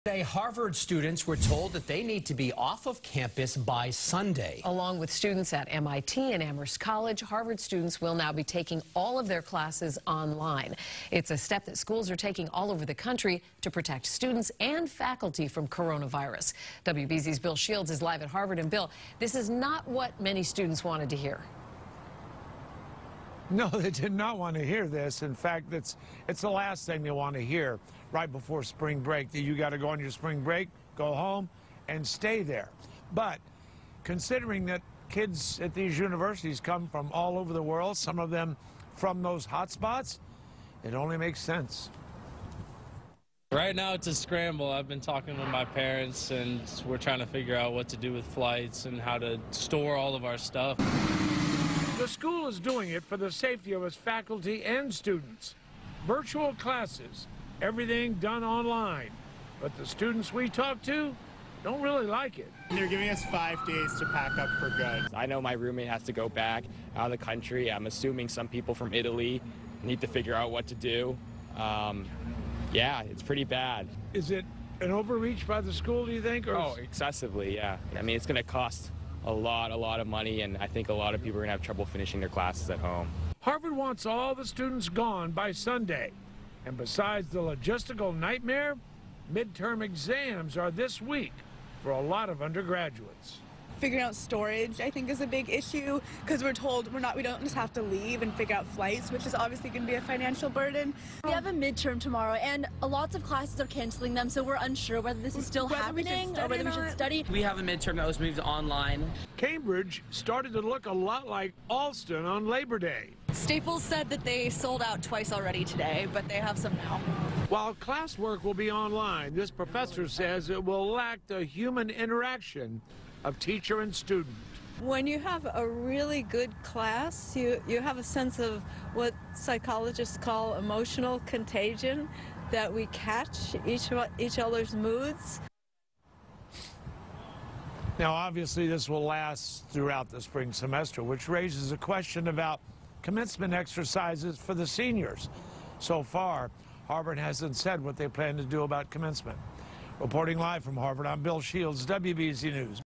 Harvard Tells Students To Move Out By Sunday Due To Coronavirus, Classes Moving Online
Harvard University students will now be taking all classes online. WBZ-TV's Bill Shields report.